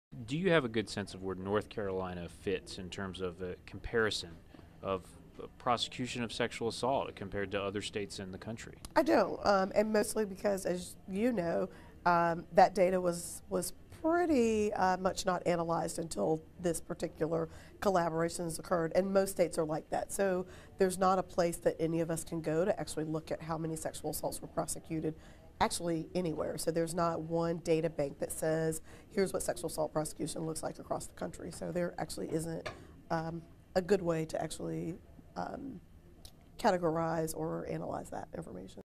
Monika Johnson Hostler. Seeking Conviction: Justice elusive for NC sexual assault survivors
Tyler Dukes and Brad Simmons/WRAL-TV

Monika Johnson Hostler, executive director for the N.C. Coalition Against Sexual Assault, sits down with WRAL-TV’s Tyler Dukes to talk about where North Carolina sits in comparison with other states when it comes to the conviction of defendants charged with sexual assault in North Carolina.

Seeking Conviction, examines the issue of sexual assault prosecution and convictions in North Carolina.